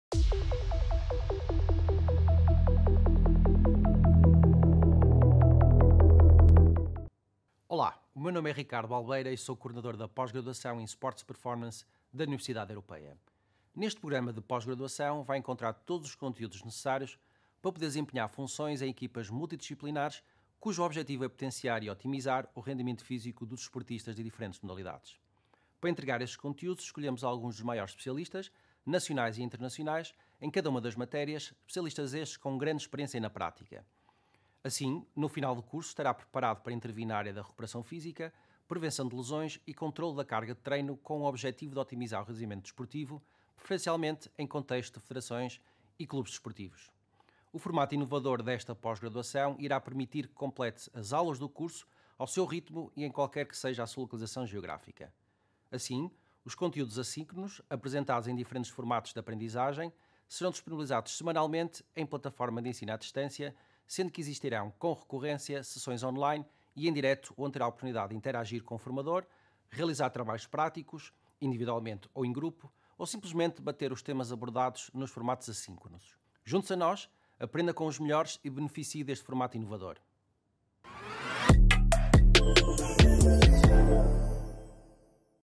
Pós-Graduação em Sports Performance
Conheça a Pós-Graduação em Sports Performance, com o Coordenador Ricardo Balbeira,.